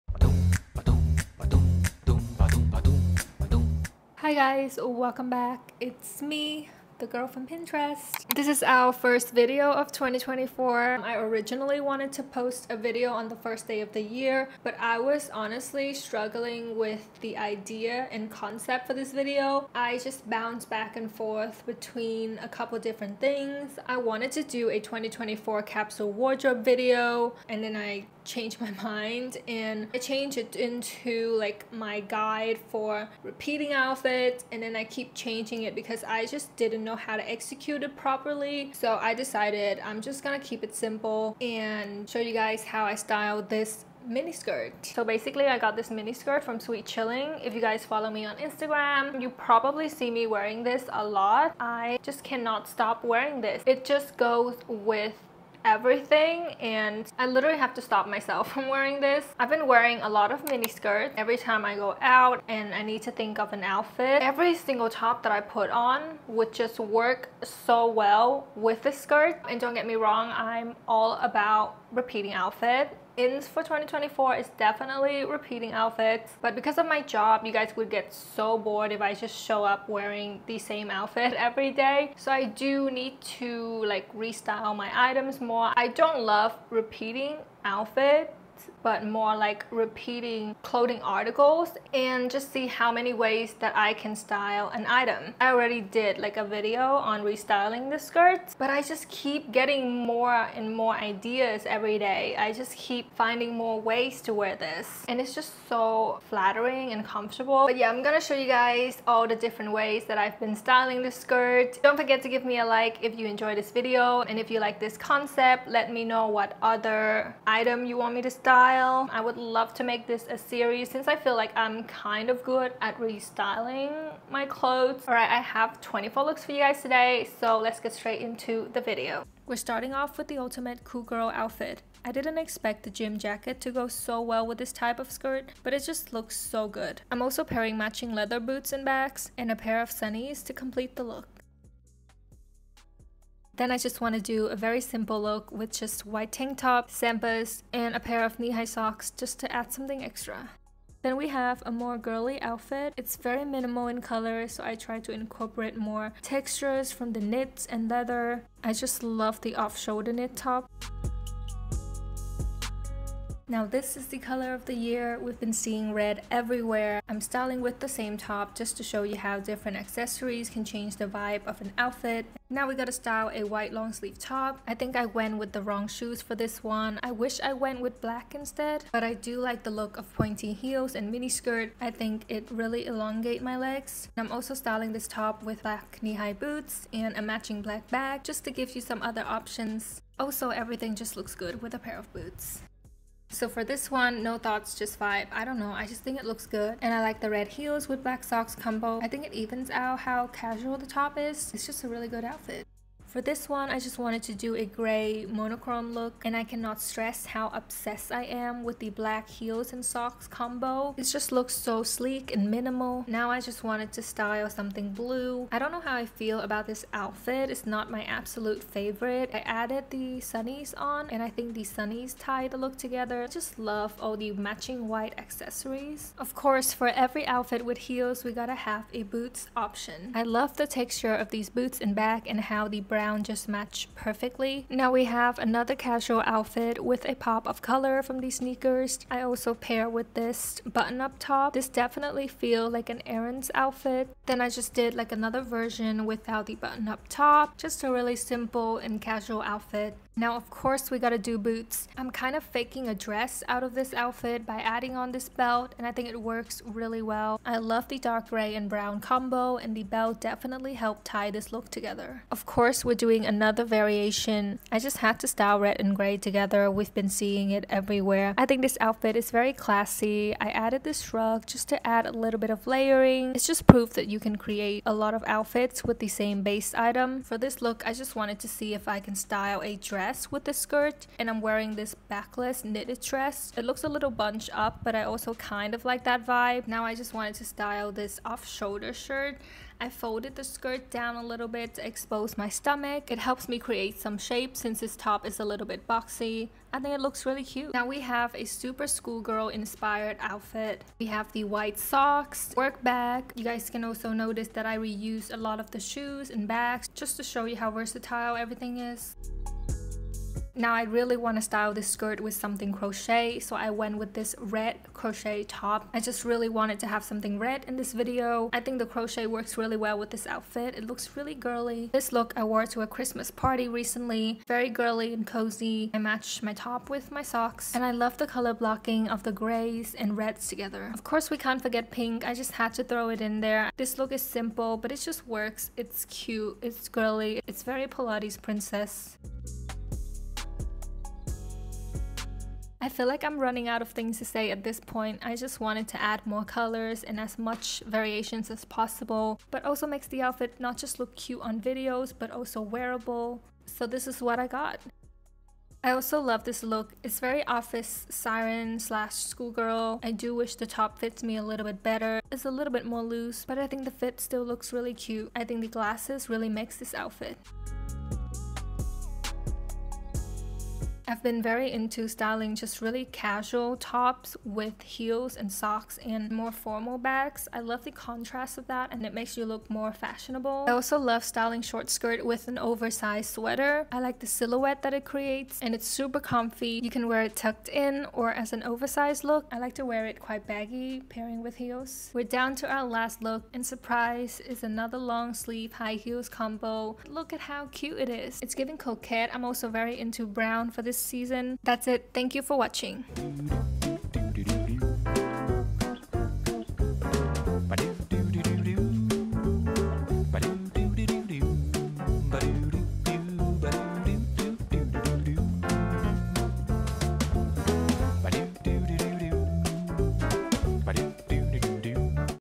How to rewear a mini skirt | 1 SKIRT 25 OUTFITS
Everything is linked below!

Hi, I'm back with another styling video! In this video I'll be showing you guys how to "repeat outfits" in a fun creative way using 1 mini skirt to create Pinterest worthy looks! Hope you love this video, ily!!!

OUTFIT #2
Top: H&m
Bag: Zara
Shoes: Adidas
OUTFIT #3
Shoes & bag: Thrifted
OUTFIT #4
Shoes & bag: Thrifted
OUTFIT #5
Bag: Thrifted
OUTFIT #6
Top: Cotton On
Shoes: Thrifted
OUTFIT #7
Bag: Hvisk
OUTFIT #8
Bag: Source Unknown store
OUTFIT #9
Top: Cotton On
Shoes: thrifted
OUTFIT #10
Top: Cotton On
Shoes: Thrifted
Bag: hvisk
OUTFIT #11
Top: Cotton On
Shirt: H&m
Shoes: Tiger Onitsuka
OUTFIT #12
Top: Cotton On
Shoes: Tiger Onitsuka
OUTFIT #13
Top: Cotton On
Shoes: Thrifted
Bag: Hvisk
OUTFIT #14:
Shrug: Cider
Bag: Thrifted
OUTFIT #15
Bag: Thrifted
OUTFIT #16
Top:
Bag: Hvisk
OUTFIT #17
OUTFIT #18
Shoes + bag: Thrifted
OUTFIT #19
Bag: Thrifted
OUTFIT #20
Top: tiger mist
OUTFIT #22
Top: H&m
Shoes: Thrifted
OUTFIT #23
Top: Raga Malak
Shoes: Thrifted
OUTFIT #24
Top: Pull & bear
Shoes: Thrifted
OUTFIT #25
Top: With Jean
Shoes: Thrifted
Bag: Hvisk